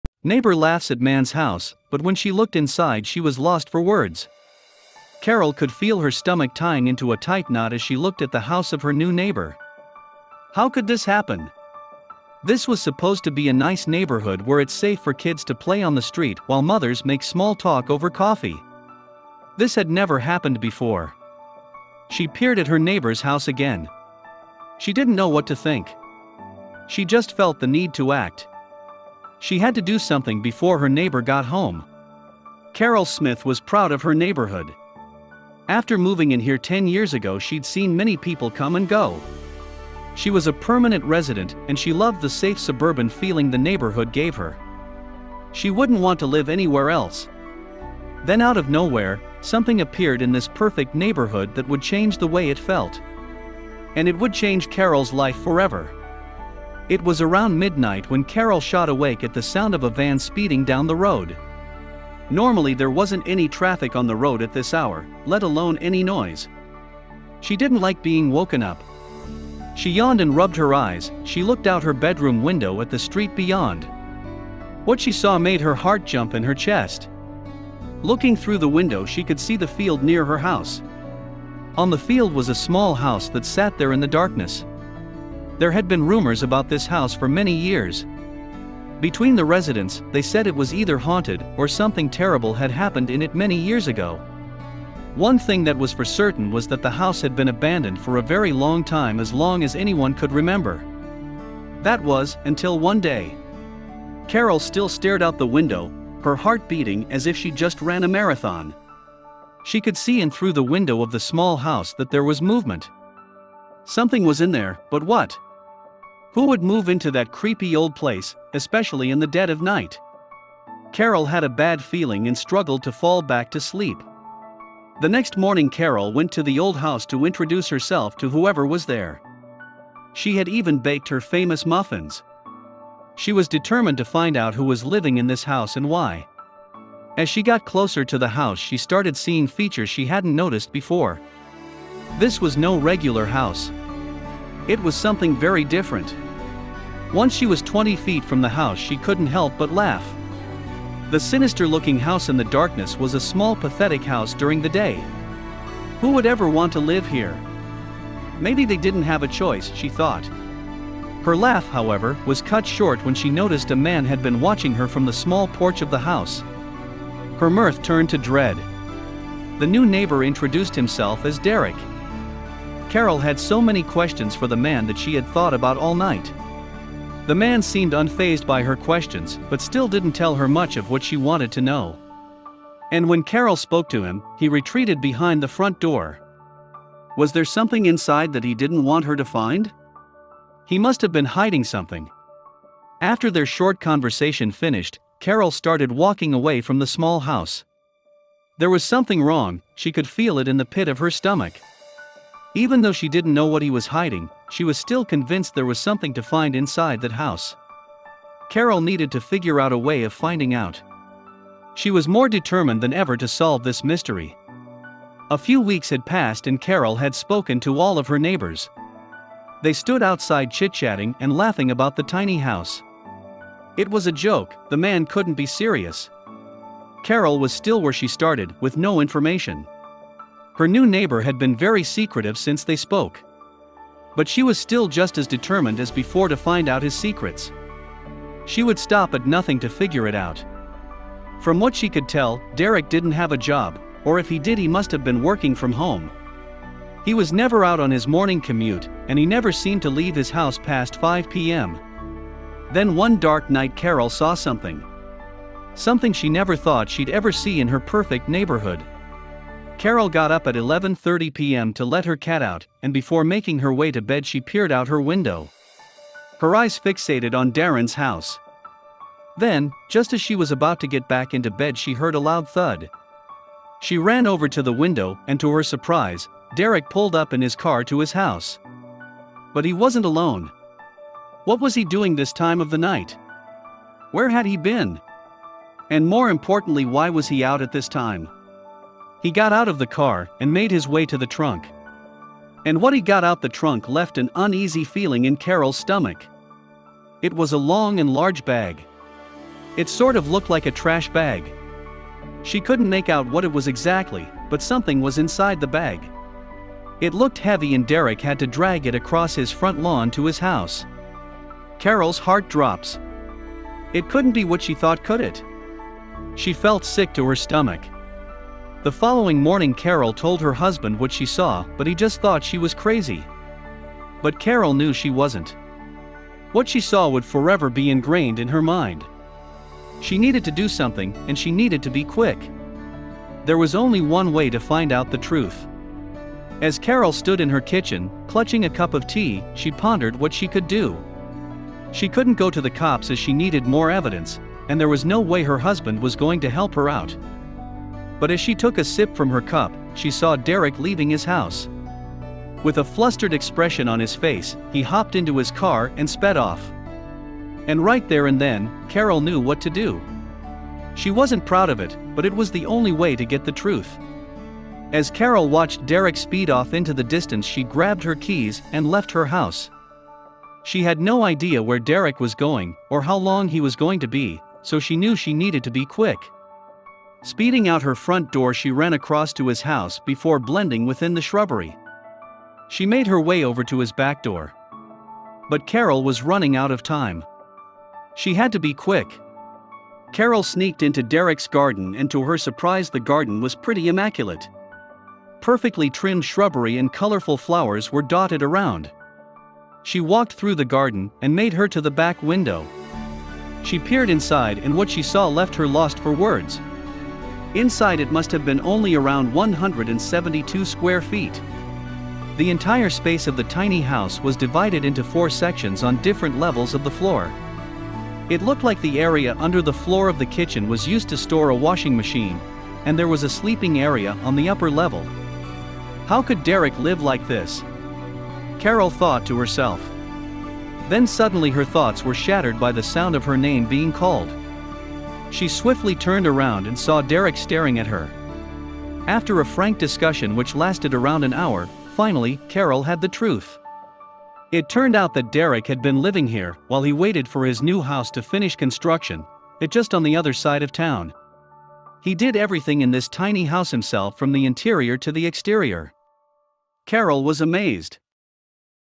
Neighbor Laughs At Man’s House, But When She Looked Inside She Was Lost For Words
Carol could feel her stomach tying into a tight knot as she looked at the house of her new neighbor. How could this happen? This was supposed to be a nice neighborhood where it’s safe for kids to play on the street while mothers make small talk over coffee. This had never happened before.

She peered at her neighbor’s house again. She didn’t know what to think. She just felt the need to act. She had to do something before her neighbor got home.